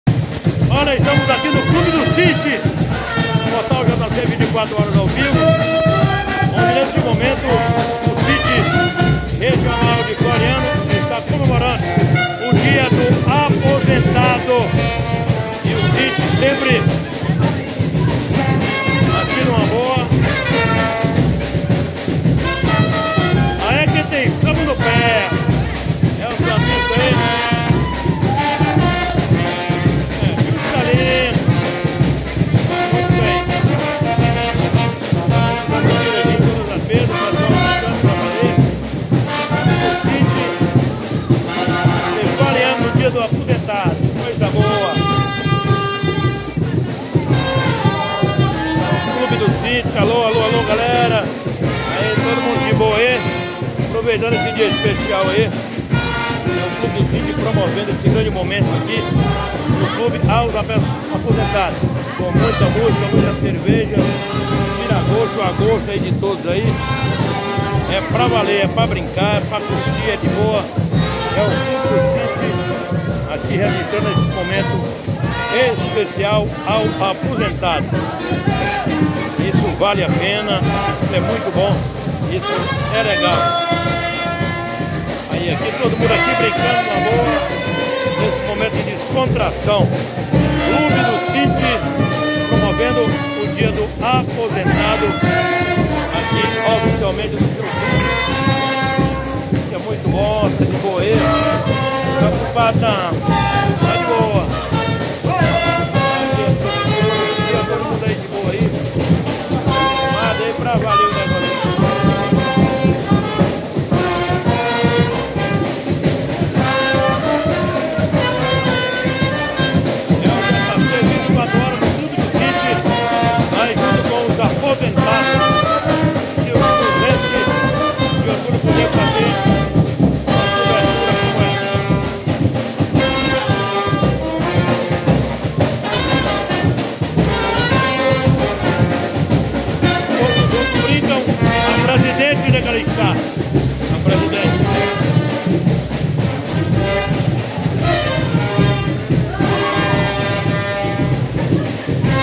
Aposentados mostraram que estão na ativa e com samba no pé comemorando seu dia
O clube do Sinte (Sindicato dos Trabalhadores em Educação) na cidade de Floriano, realizou no último sábado, um evento festivo para comemorar o dia nacional do aposentado, com a participação de pessoas que já deram a sua parcela de contribuição nas mais diversas áreas de trabalho e hoje gozam do direito de se divertir sem ter que ficar olhando para o relógio pensando no expediente do dia seguinte.
Com muita alegria, descontração e samba no pé, os melhores momentos da festa dessa categoria pessoas especiais e guerreiras, foram registrados pelo Repórter Carlos Iran, para o Portal jc24horas. Confira.